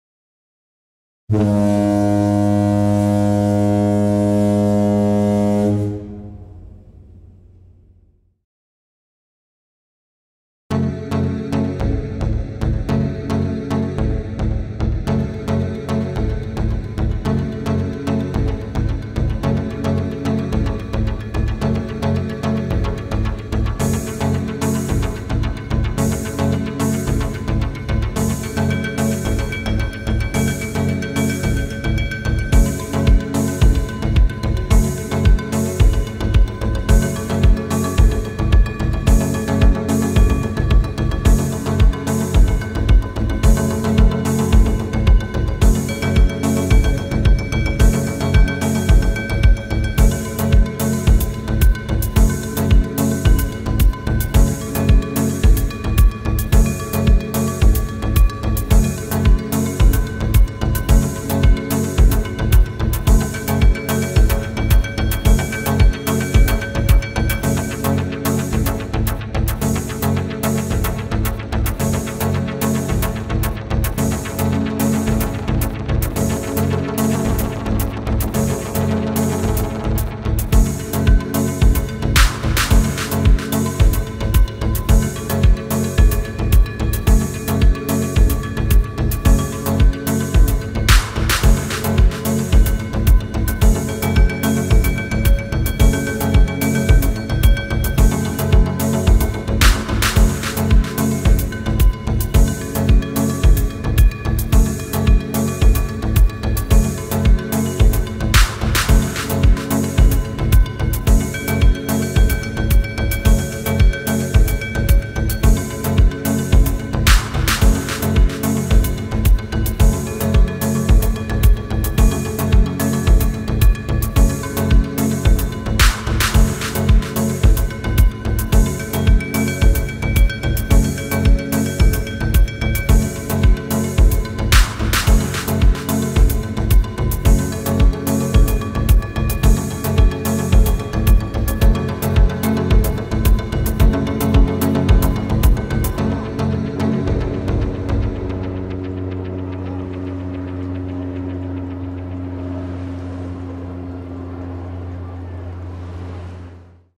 PACIFIC Tugboat Seaspan Bunker Barge 2022
Port of Vancouver Canada we see SEASPAN tugs with fuel Barge for the ocean liner Queen Elizabeth  in the Summer 2022 c LSAC